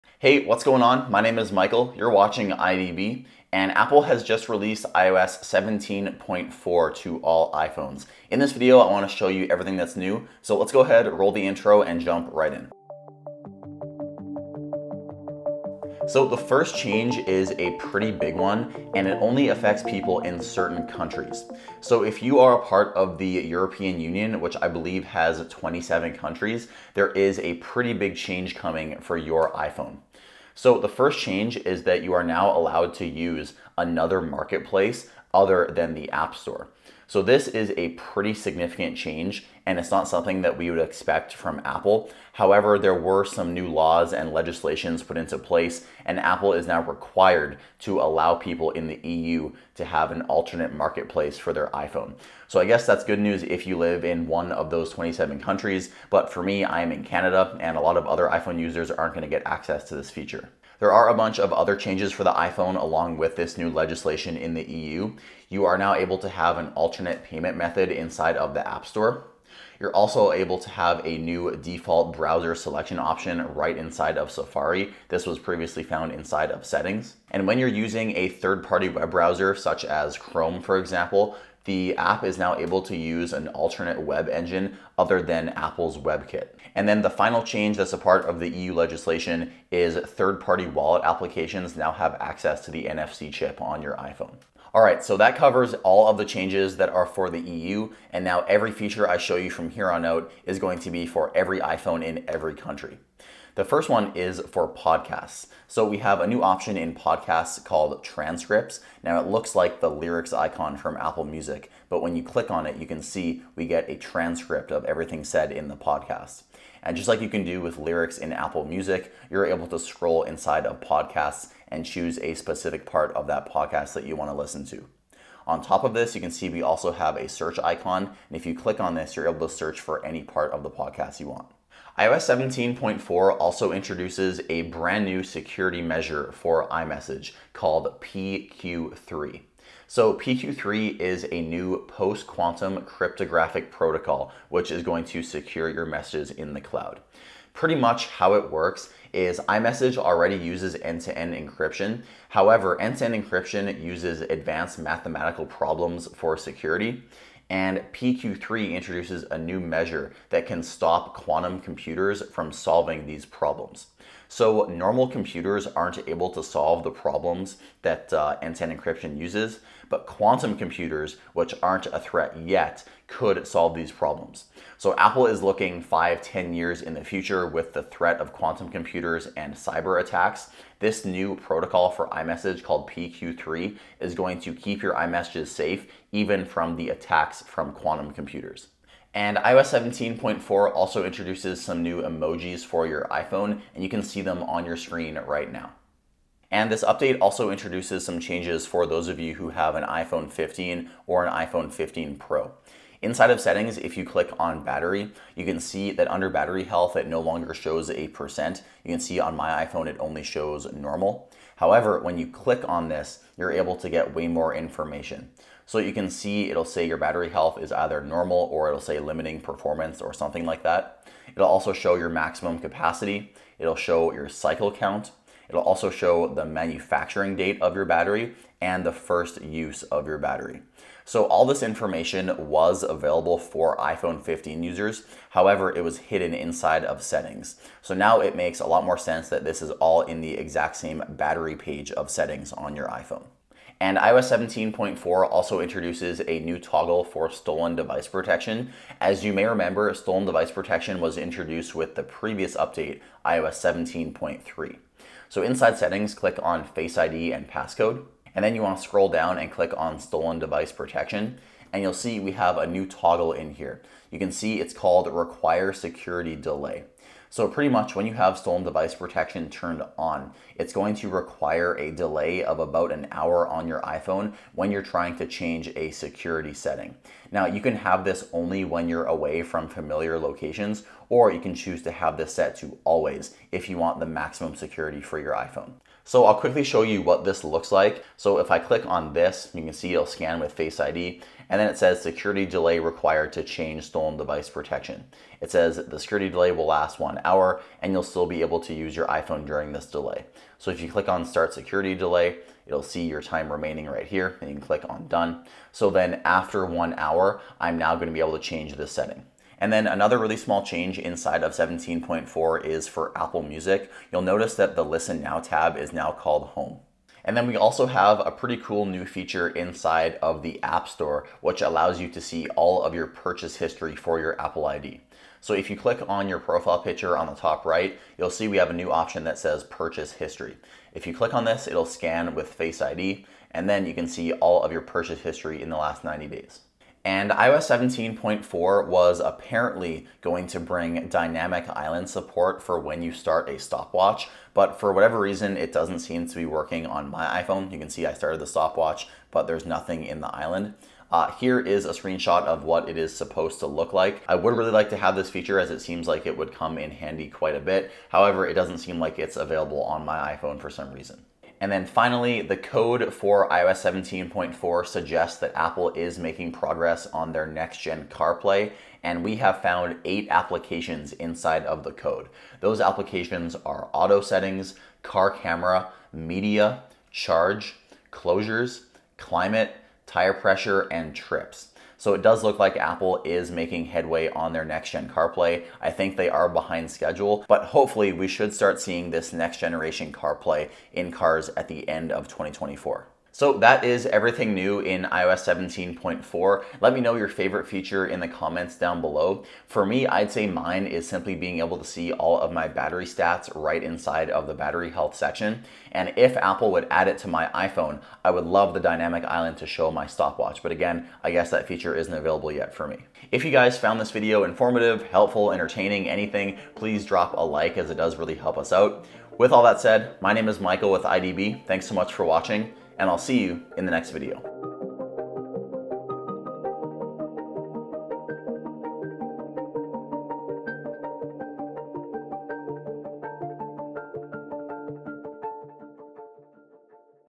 iOS 17.4 Released - Here's Everything New!
iOS 17.4 is now available with some changes for your iPhone. This update introduces the option for iPhone users in the EU to use an alternate app marketplace. This update also introduces new emojis, podcast transcripts, an updated battery health screen, and more!